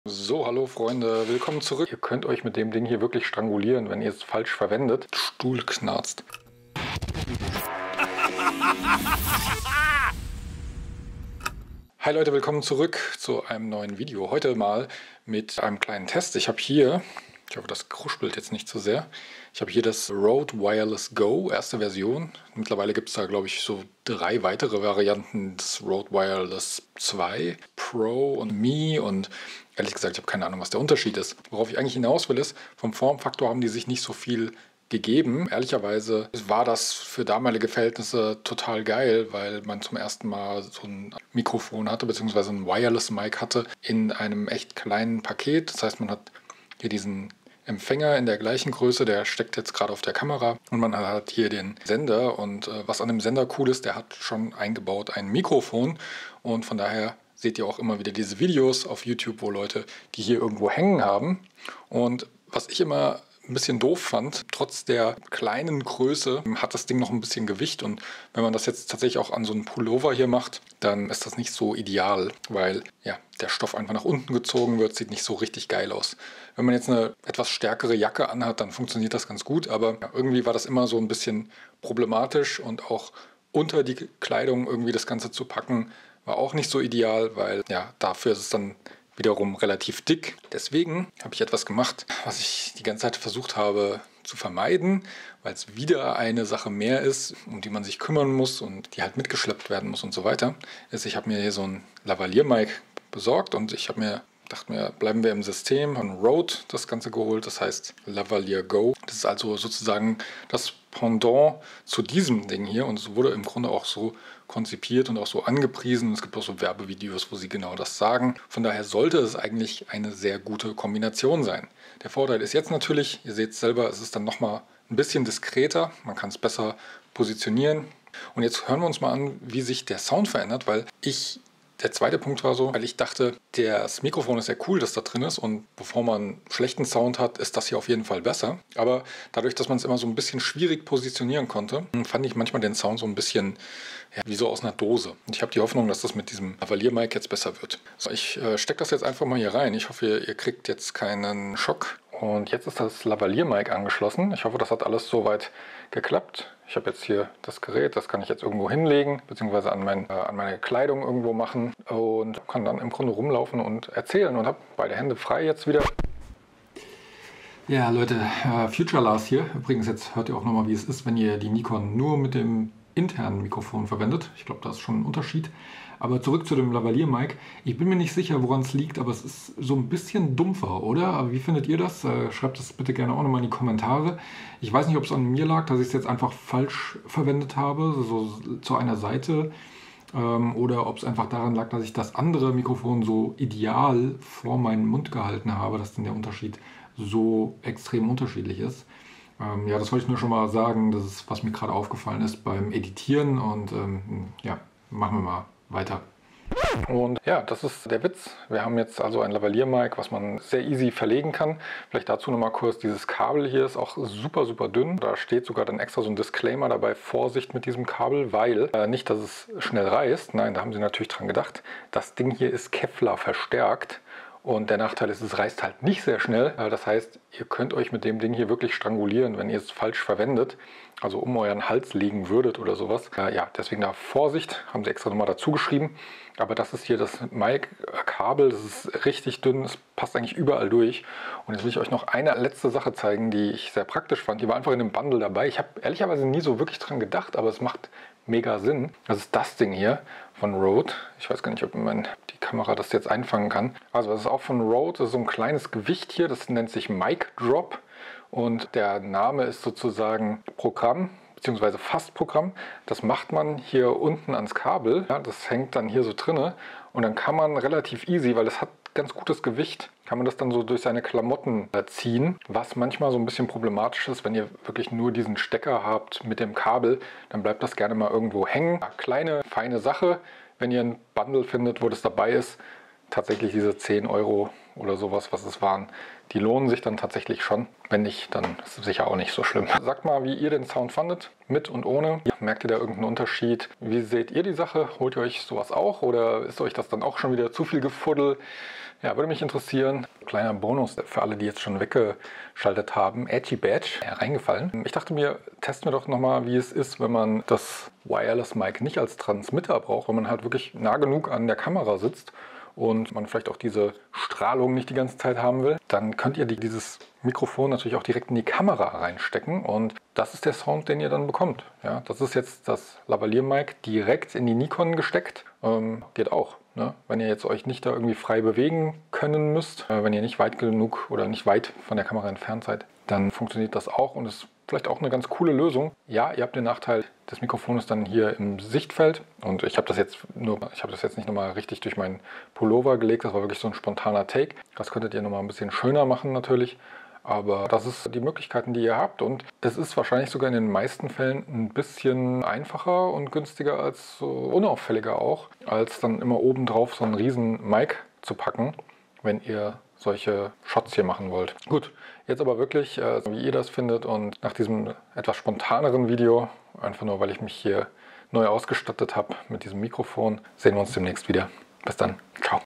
Lav Mic = Love Mic?
In diesem Video zeige ich euch, wie ihr ein Lavaliermikrofon an der Nikon Z30 anschließen könnt. Das Lavaliermikrofon ist ein kleines, unauffälliges Mikrofon, das ihr an eurer Kleidung befestigen könnt.

Wie gut das ganze bei mir funktioniert, hört ihr in diesem Video!